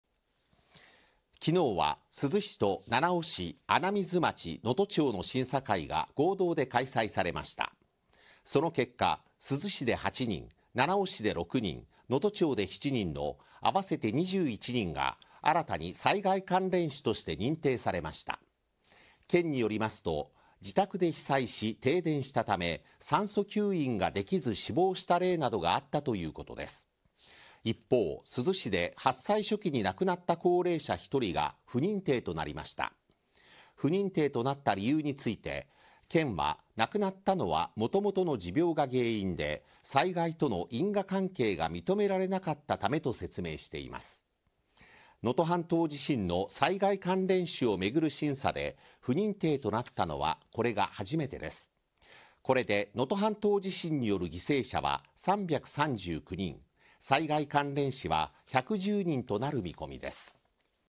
能登半島地震の災害関連死初めての不認定…新たに21人が認定 (2024年7月31日)
能登半島地震による災害関連死を認定する合同の審査会が行われ、21人が新たに認定されました。

珠洲市と七尾市、穴水町、能登町の審査会が合同で開催されました。その結果、珠洲市で8人、七尾市で6人、能登町で7人の、あわせて21人が新たに災害関連死として認定されました。県によりますと自宅で被災し停電したため酸素吸入ができず死亡した例などがあったということです。

一方、珠洲市で発災初期に亡くなった高齢者1人が不認定となりました。不認定となった理由について県は、亡くなったのは元々の持病が原因で災害との因果関係が認められなかったためと説明しています。能登半島地震の災害関連死を巡る審査で不認定となったのはこれが初めてです。これで能登半島地震による犠牲者は339人、災害関連死は110人となる見込みです。